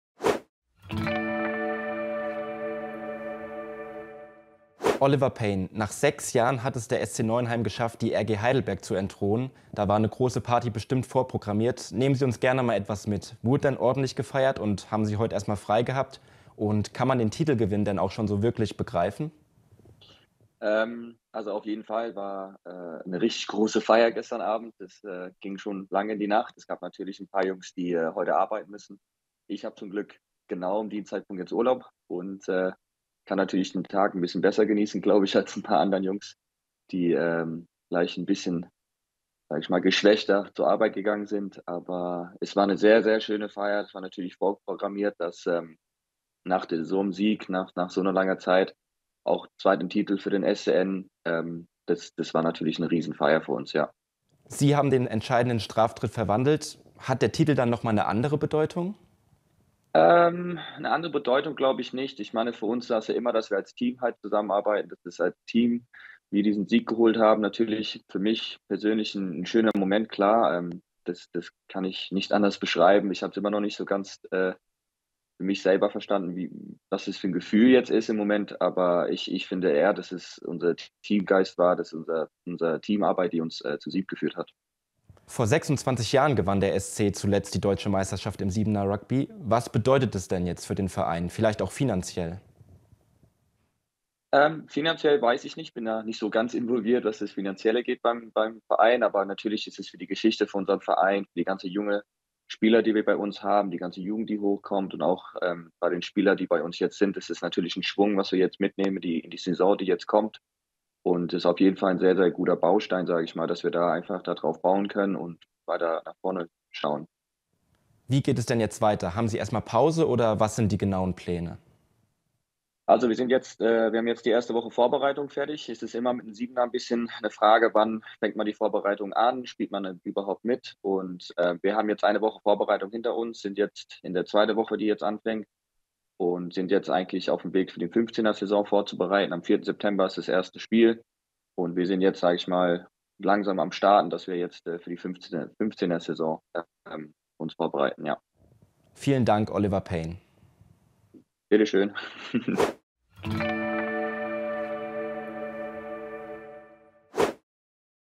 SC Neuenheim ist Deutscher Meister im 7er Rugby | RON TV
Große Freude gab es am Wochenende in Heidelberg: Der SC Neuenheim konnte sich in einem packenden Finale mit 13:10 nach Verlängerung zum Deutschen Meister im 7er-Rugby küren. Unser Reporter Christopher Vogel hat mit Kapitän Oliver Paine über den Titel und die Feierlichkeiten gesprochen.